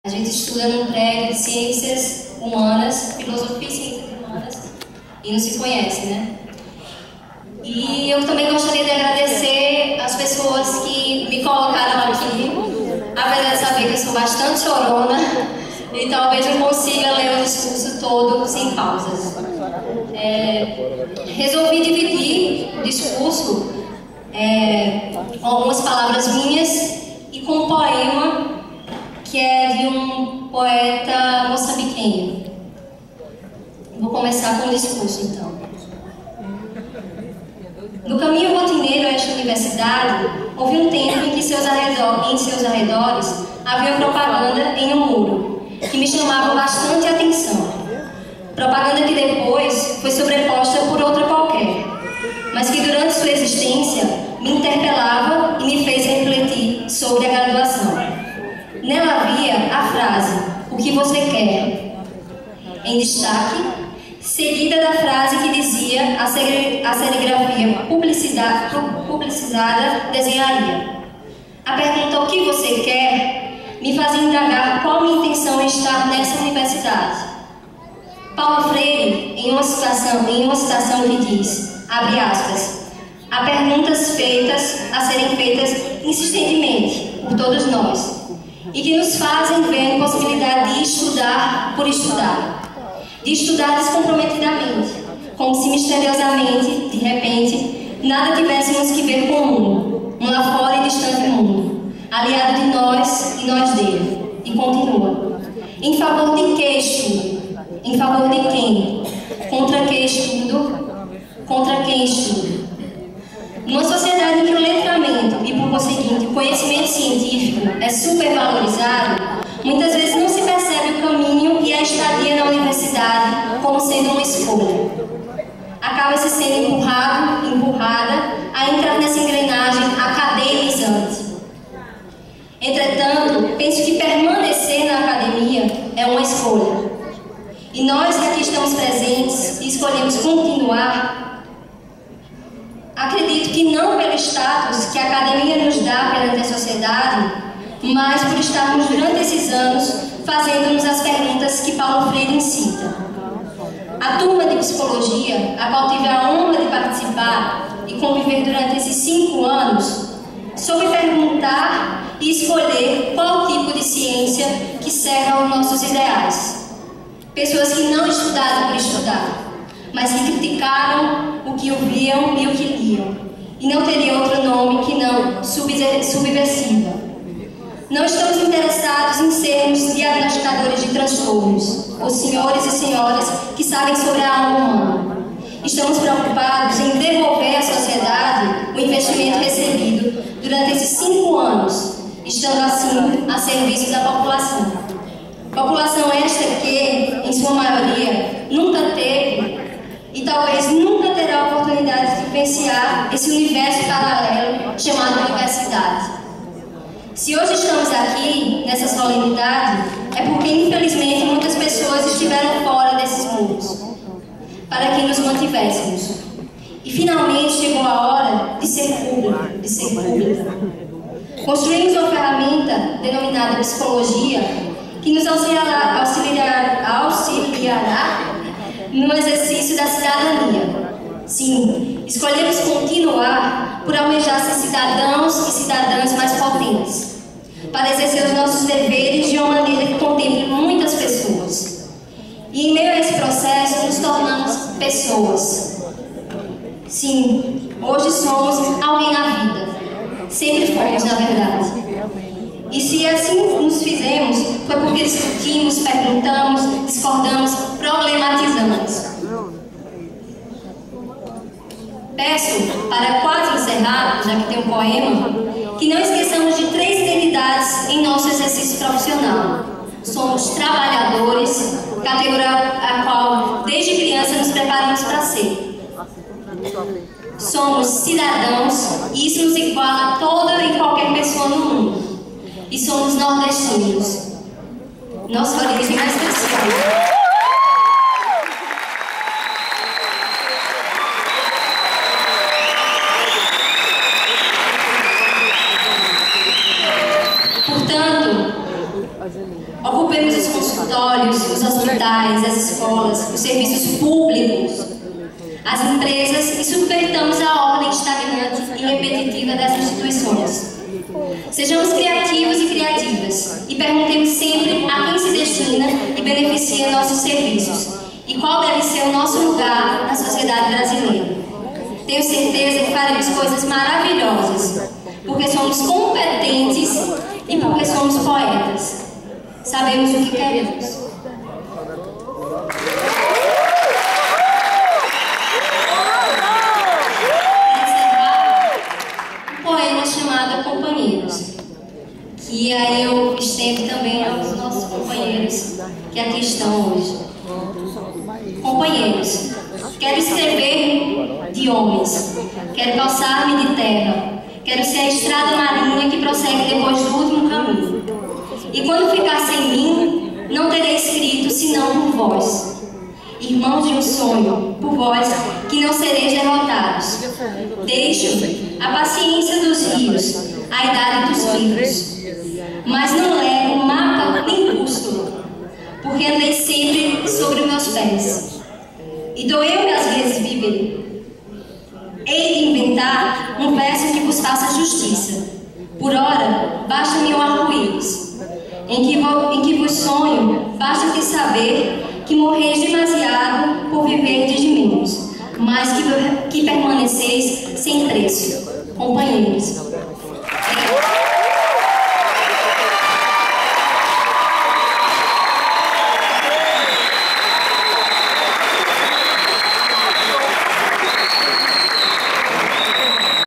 Formatura de Psicologia - UFPE - 2011.2
Oradora da turma de Psicologia UFPE, 2011.2 - Anita Vitorino. Foi o melhor discurso da noite! =)